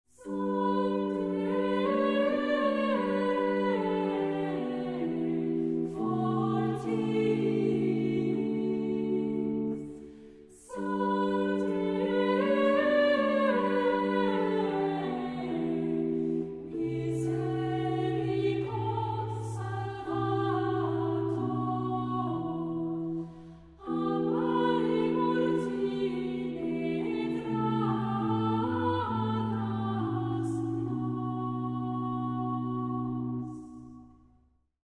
Tranen van Brabant teaser - Holland Baroque
Holland Baroque presenteert samen met vijf vrouwelijke solisten de muzikale tradities van het 17de-eeuwse kloosterleven in Noord-Brabant.
In dit programma hoort u een vrouwenkoor met passiemuziek zoals die in Brabantse kloosters geklonken heeft.

Holland Baroque presents together with five female soloists the musical traditions of 17th century convent life in province North-Brabant, The Netherlands. In this program you will hear a female choir singing Passion music as it was heard in Brabant convents.